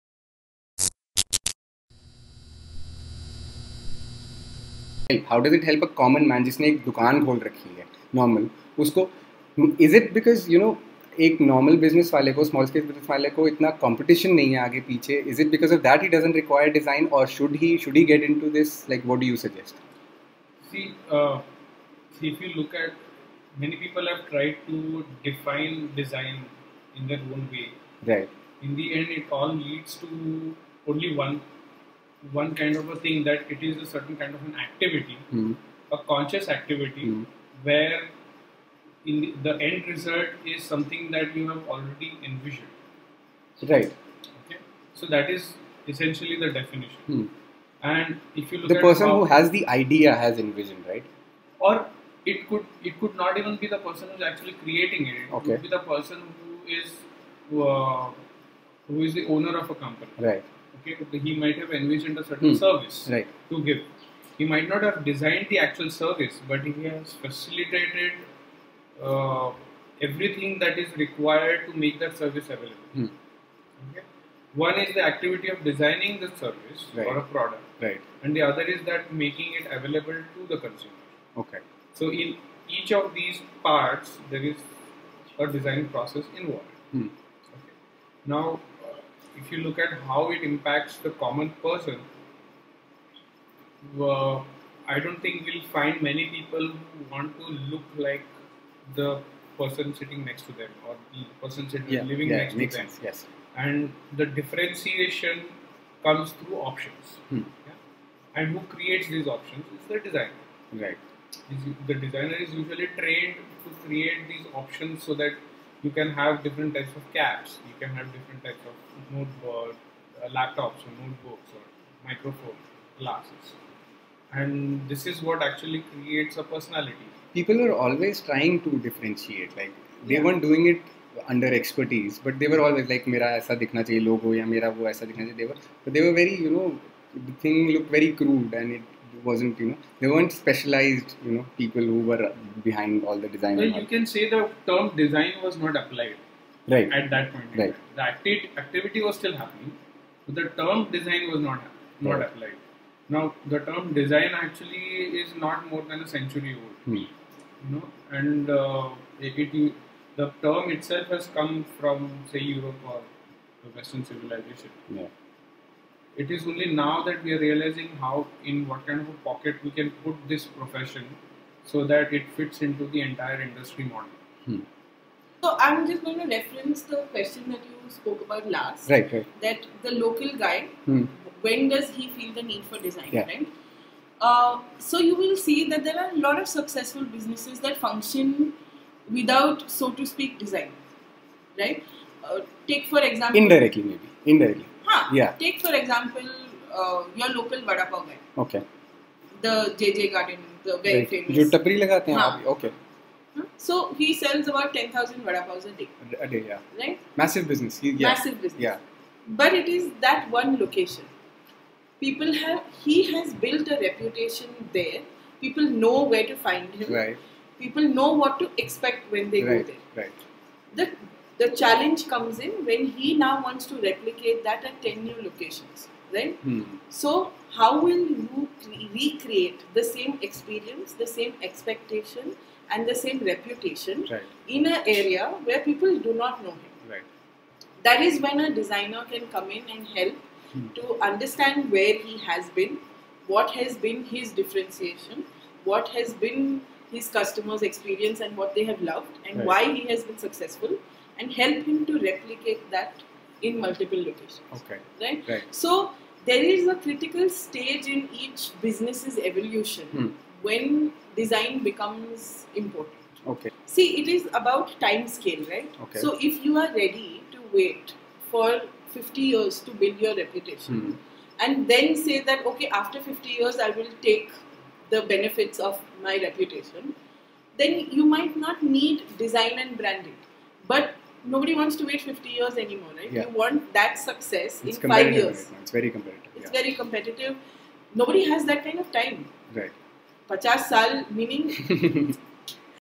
HIGHLIGHTS: Is DESIGN the key to a successful business?
Brand strategist Dhun Patel and award winning product designer Nitin Virkar talk to Abhimanyu about how a design and branding can be a game changer for any business. Dhun and Nitin are co-founders of an award wining design firm in Pune called Therefore design.